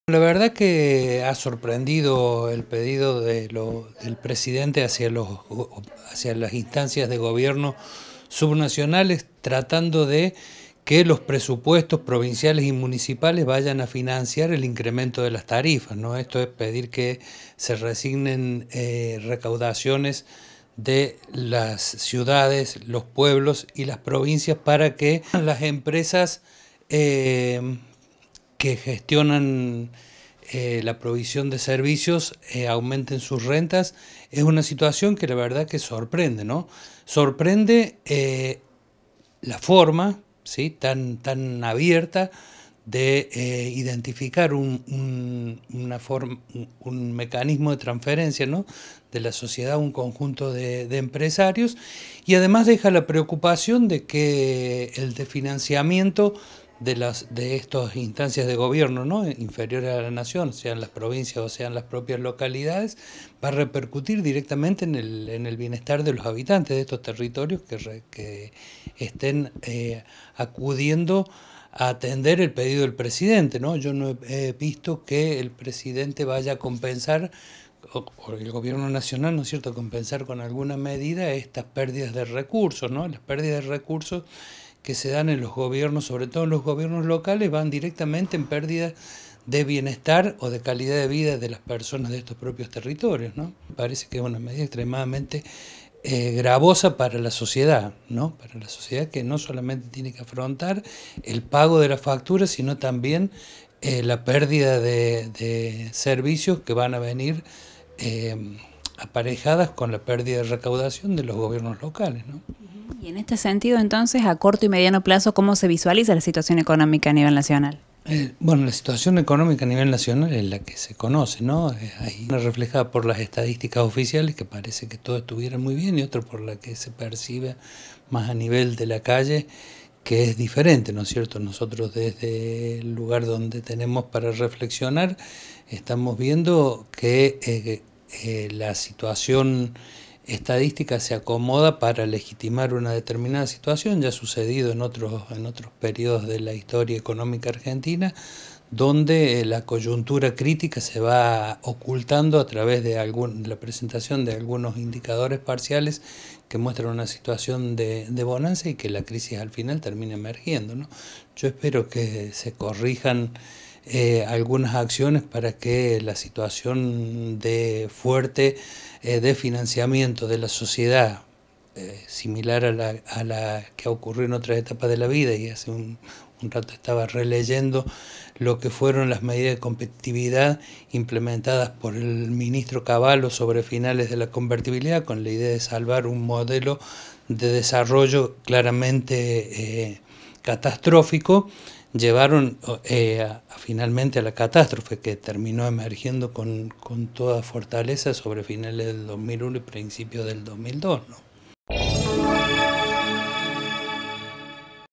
AUMENTO DE TARIFAS - JORGE HERNÁNDEZ
EL ANÁLISIS DE LAS MEDIDAS ADOPTADAS POR EL GOBIERNO NACIONAL
Facebook.com/uniriotv
Twitter.com/uniriotv